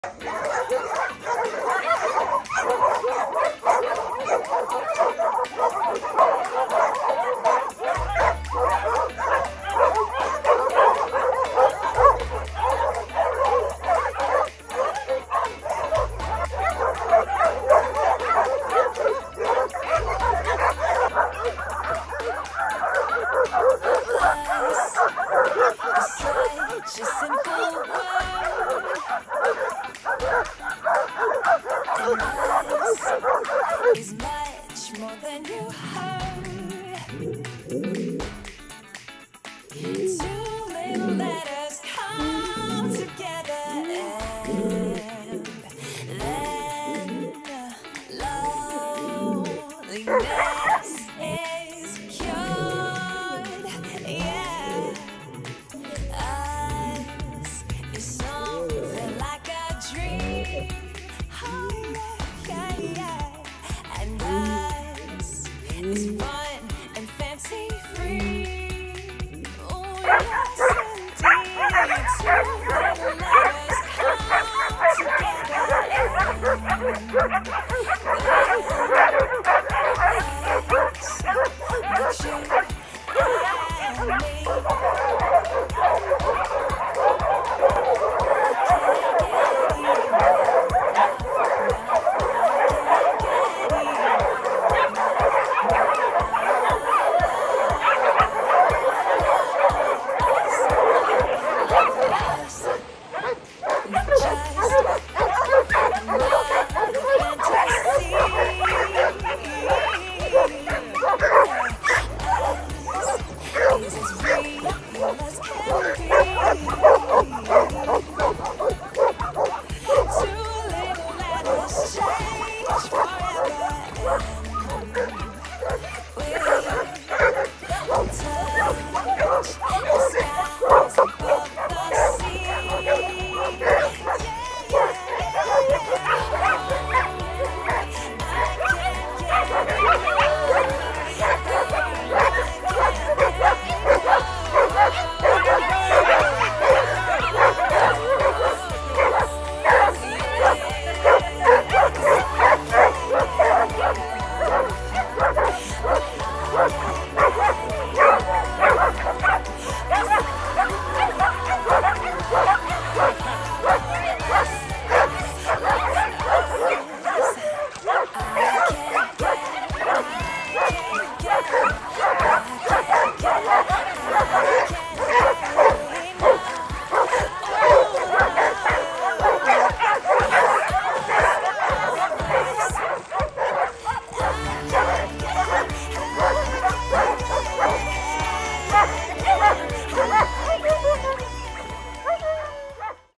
The Therapy Pack Letting Loose
Time to let loose with our favourite pool games; lure off the dock and swimming after the big ball. Lunchtime and the pool session is always going to be our time for the therapy pack to let loose. No training sessions, just a whole lot of fun.